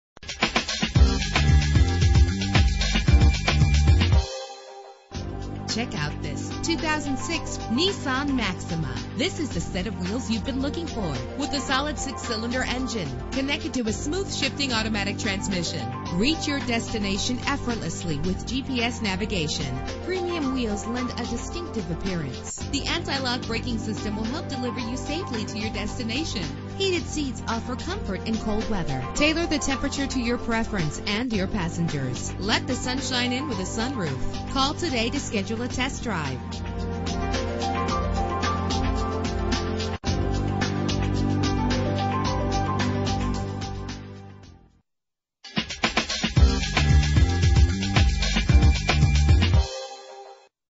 2006 Nissan Maxima - Orem UT
Year: 2006
Make: Nissan
Model: Maxima
Trim: SL
Engine: 3.5 liter 6 cylinder 24 valve
Transmission: Automatic
Color: Gold
Mileage: 53501
Address:
285 W Univeristy Parkway